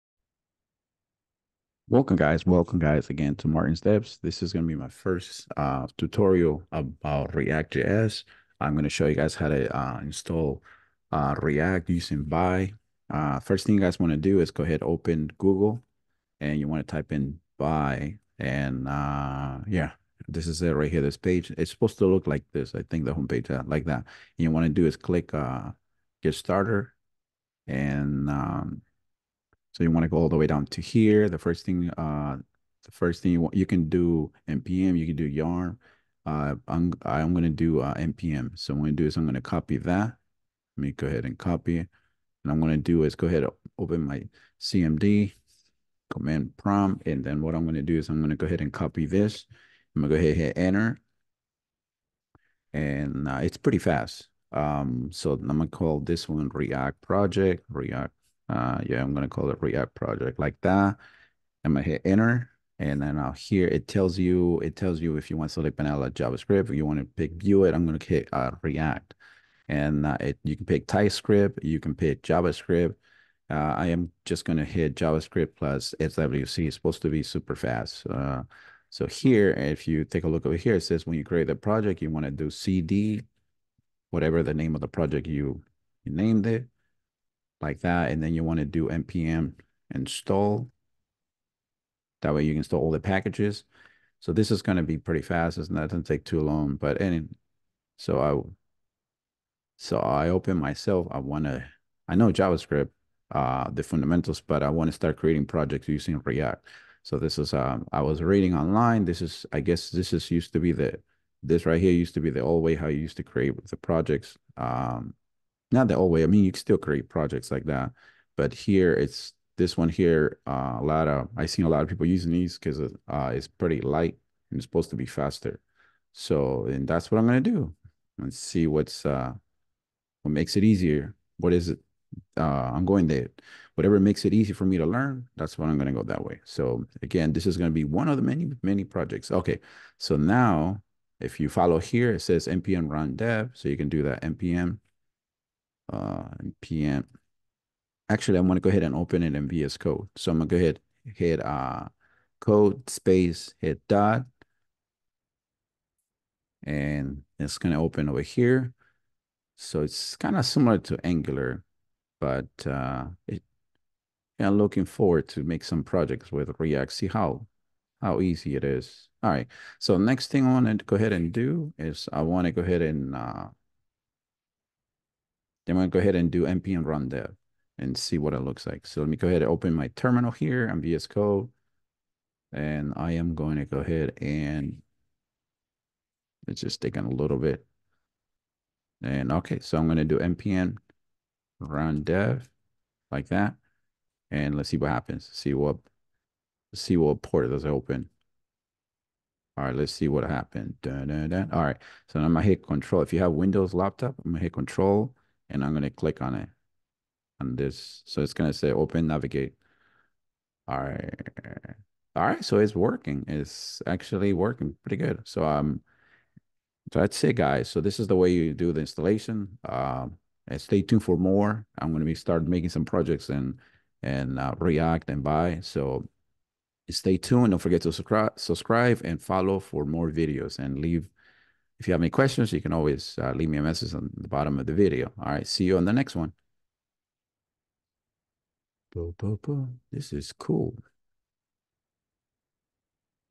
Set up a vite + react + Javascript project (2024)
Welcome to our step-by-step tutorial on setting up a modern web development environment using Vite, React, and TypeScript in 2024! In this video, we'll walk you through the process of creating a robust foundation for your web applications, leveraging the speed and efficiency of Vite, the power of React for building dynamic user interfaces, and the type safety of TypeScript.

Whether you're a seasoned developer looking to explore the latest tools or a newcomer eager to dive into the world of web development, this guide is designed to help you get started quickly and confidently. Follow along as we cover everything from project initialization to configuring dependencies, setting up TypeScript, creating React components, and running your project locally.

By the end of this tutorial, you'll have a fully functional Vite + React + TypeScript project ready to customize and expand according to your unique requirements. Say goodbye to tedious setup processes and hello to streamlined development with the latest technologies.

Don't miss out on this opportunity to level up your web development skills and stay ahead of the curve in 2024. Hit play now and let's get started on building amazing web experiences together!

Chapters
00:00 Introduction to React Installation 📚
00:07 Opening Google and Searching for "React" 🔍
00:32 Copying and Running the N Command ⚙️
01:03 Selecting JavaScript and SSWC 🛠
01:21 Instruction to CD and NPM 📥
02:18 Choosing the Easier Way to Learn React ⚡
02:50 Opening the Project in VS Code 🖥️
03:03 Running 'NPN Run Dev' Command 🏃♂️
03:26 Opening the in the Browser 🌐
03:34 Successful React Installation ✅
03:56 Conclusion and Call to Action ✉

🔨 Tools I used :
° JavaScript
° Vite + React
° HTML5
° CSS3